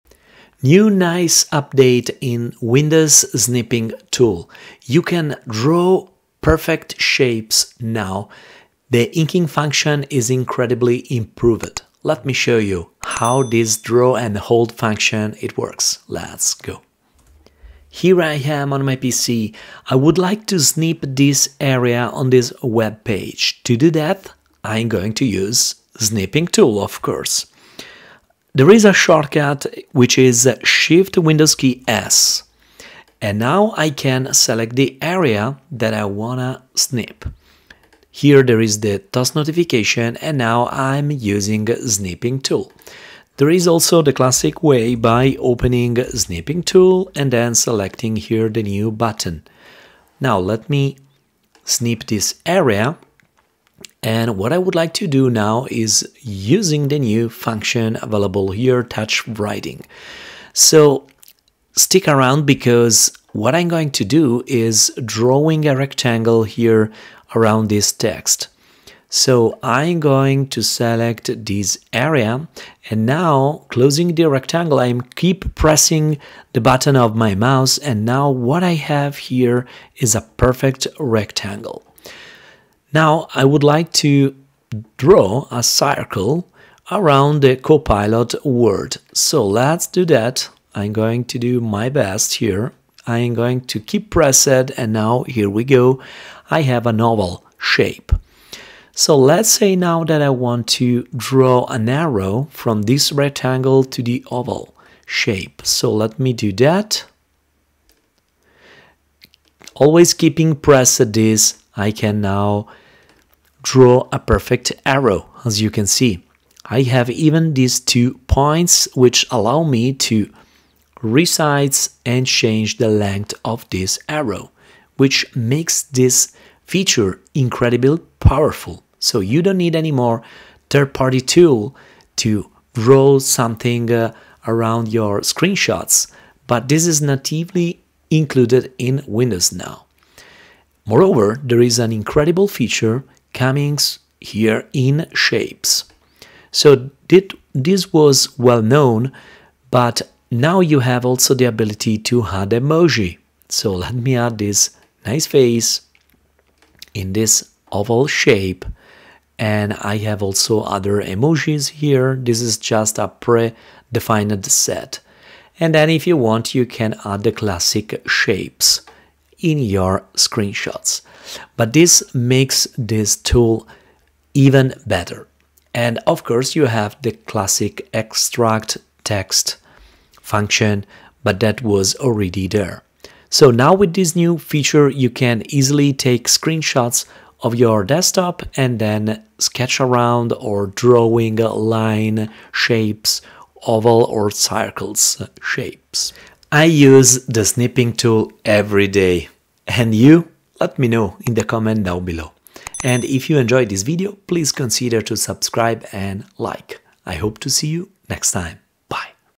Windows Snipping Tool Tips: Neat Lines & Shapes with Draw & Hold
Discover the latest update to the Windows Snipping Tool with the new 'Draw & Hold' feature!
In this video, I'll show you how to use this exciting addition to create perfect shapes and lines effortlessly. Whether you're marking up screenshots for work or adding annotations for personal projects, 'Draw & Hold' makes it easier than ever to achieve professional-looking results. Join me as we explore step-by-step instructions, tips, and tricks to make the most of this powerful tool.

⏲Timestamps
00:00 Introduction
00:17 Draw & Hold
02:22 Add Emojis'
03:16 Conclusion